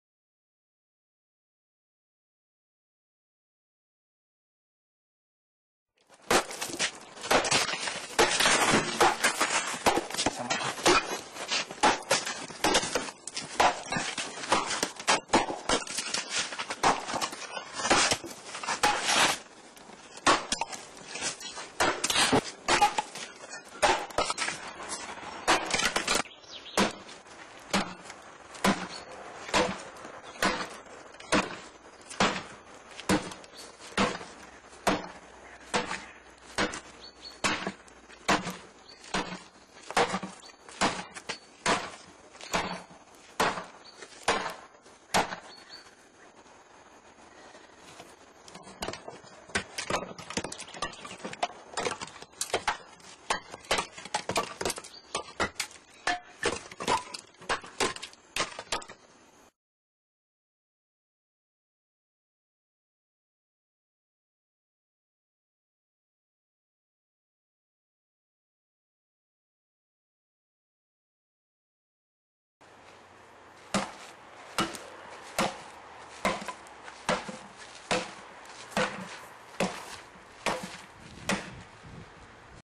The prototype of the Leveraxe
Leveraxe is a precision tool for splitting firewood. Have fun and split your firewood fast and safely.